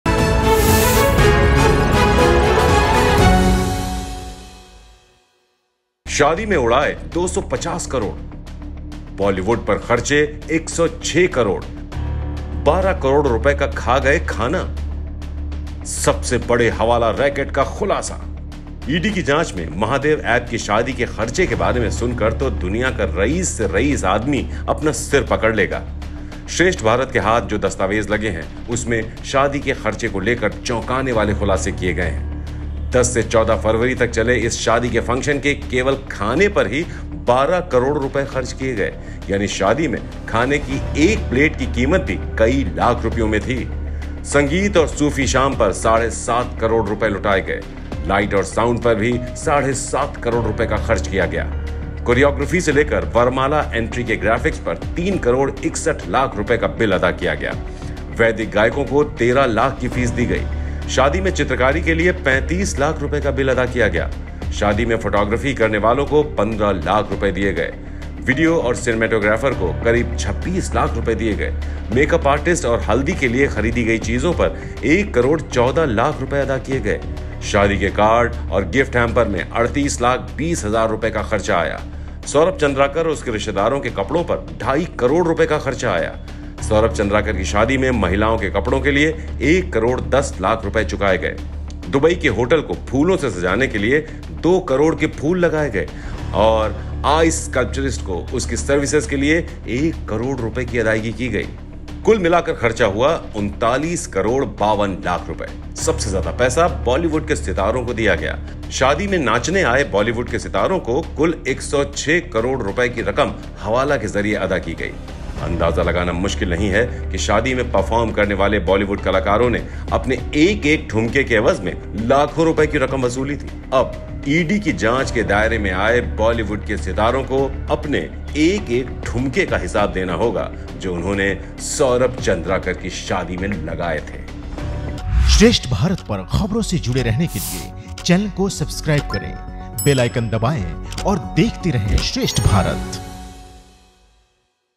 SAURABH CHANDRAKAR की शादी में खाने की एक प्लेट की कीमत जानकर चौंक जाएंगे आप| Hindi News
ED की जांच में महादेव ऐप की शादी के खर्चे के बारे में सुनकर तो दुनिया का रहीस से रहीस आदमी अपना सिर पकड़ लेगा. श्रेष्ठ भारत के हाथ जो दस्तावेज लगे हैं उसमें शादी के खर्चे को लेकर चौंकाने वाले खुलासे किए गए हैं. 10 से 14 फरवरी तक चले इस शादी के फंक्शन के केवल खाने पर ही 12 करोड़ रुपये खर्चे गए यानि शादी में खाने की एक प्लेट की कीमत भी कई लाख रुपयों में थी. संगीत और सूफी शाम पर साढ़े सात करोड़ रुपये लुटाए गए. लाइट और साउंड पर भी साढ़े सात करोड़ रुपये का खर्चा किया गया. कोरियाग्राफी से लेकर वरमाला एंट्री के ग्राफिक्स पर 3 करोड़ 61 लाख रुपये का बिल अदा किया गया. वैदिक गायकों को 13 लाख की फीस दी गई. शादी में चित्रकारी के लिए 35 लाख रुपये का बिल अदा किया गया. शादी में फोटोग्राफी करने वालों को 15 लाख रुपये दिए गए. वीडियो और सिनेमेटोग्राफर को करीब 26 लाख रुपये दिए गए. मेकअप आर्टिस्ट और हल्दी के लिए खरीदी गई चीजों पर 1 करोड़ 14 लाख रुपये अदा किए गए. शादी के कार्ड और गिफ्ट हैंपर में 38 लाख 20 हजार रुपये का खर्चा आया. सौरभ चंद्राकर और उसके रिश्तेदारों के कपड़ों पर ढाई करोड़ रुपये का खर्चा आया. सौरभ चंद्राकर की शादी में महिलाओं के कपड़ो के लिए 1 करोड़ 10 लाख रुपये चुकाए गए. दुबई के होटल को फूलों से सजाने के लिए करीब 2 करोड़ के फूल लगाए गए और ICE SCULPTURIST को उसकी सर्विसेज के लिए 1 करोड़ रुपये की अदायगी की गई. कुल मिलाकर खर्चा हुआ 39 करोड़ 52 लाख रुपये. सबसे ज्यादा पैसा बॉलीवुड के सितारों को दिया गया. शादी में नाचने आए बॉलीवुड के सितारों को कुल 106 करोड़ रुपये की रकम हवाला के जरिए अदा की गई. अंदाजा लगाना मुश्किल नहीं है कि शादी में PERFORM करने वाले बॉलीवुड कलाकारों ने अपने एक-एक ठुमके के एवज में लाखों रुपयों की रकम वसूली थी. अब ईडी की जांच के दायरे में आए बॉलीवुड के सितारों को अपने एक-एक ठुमके का हिसाब देना होगा जो उन्होंने सौरभ चंद्राकर की शादी में लगाए थे.

The poorest of the poor in the world will be shocked to hear about the expenses incurred on Mahadev App's wedding in the ED investigation. Shocking revelations have been made regarding the wedding expenses in the documents which have been obtained by Shrestha Bharat. In this wedding function which lasted from 10th to 14th February, Rs 12 crores were spent on food alone, which means the cost of even one plate of food at the wedding was several lakhs of rupees. Seven and a half crore rupees were spent on music and Sufi evening. An expenditure of Rs 7.5 crore was also spent on lights and sound. A bill of Rs 3 crore 61 lakh was paid for everything from choreography to graphics of varmala entry. Vedic singers were given a fee of Rs 13 lakh. A bill of Rs 35 lakh was paid for painting at the wedding. Those who did photography at the wedding were given Rs 15 lakh. The video and cinematographer were given around Rs 26 lakh. Rs 1 crore 14 lakh was paid for things purchased for makeup artists and turmeric. The expenditure on wedding cards and gift hamper was Rs 38 lakh 20 thousand. An expenditure of Rs 2.5 crore was incurred on the clothes of Saurabh Chandrakar and his relatives. Rs 1 crore 10 lakh was paid for women's clothes for Saurabh Chandrakar's wedding. About Rs 2 crore flowers were planted to decorate the Dubai hotel with flowers and Rs 1 crore was paid to ICE SCULPTURIST for his services. The total expenditure was Rs 39 crore 52 lakh. The highest amount was given to Bollywood stars. A total amount of Rs 106 crore was paid through hawala to the Bollywood stars who came to dance at the wedding. It is not difficult to guess that the Bollywood actors who performed at the wedding had collected lakhs of rupees for each dance. Now the Bollywood stars who have come under the scanner of ED will have to give an account of each and every move that they made at Saurabh Chandrakar's wedding.